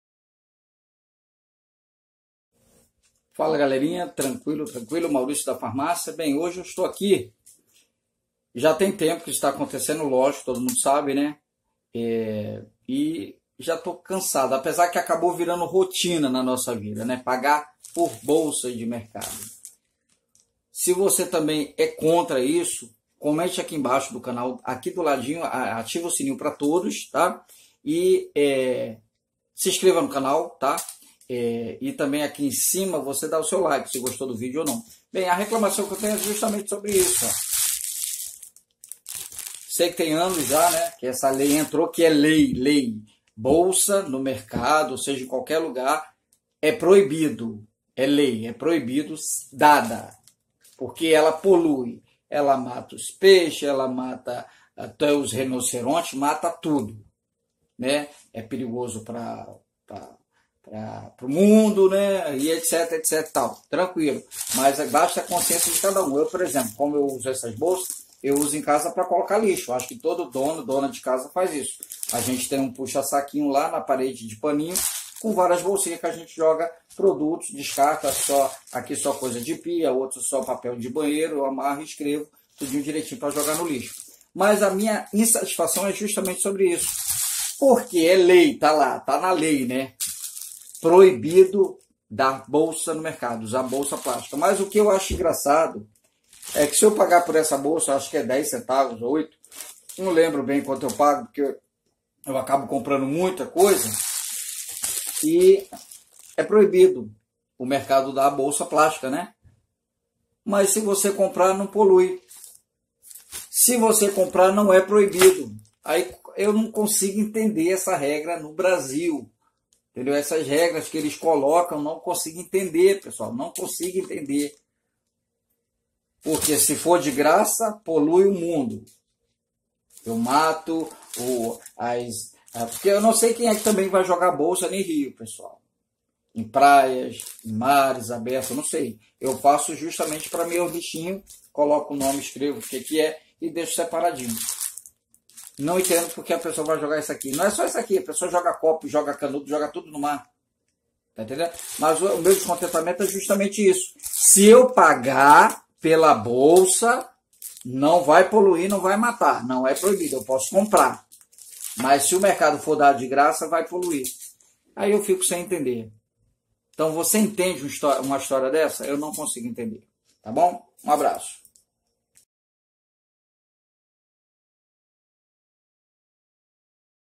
sacola paga em supermercado não polui. saiba porque. e qual é a minha reação.
é Lei paga não polui .de graça não polui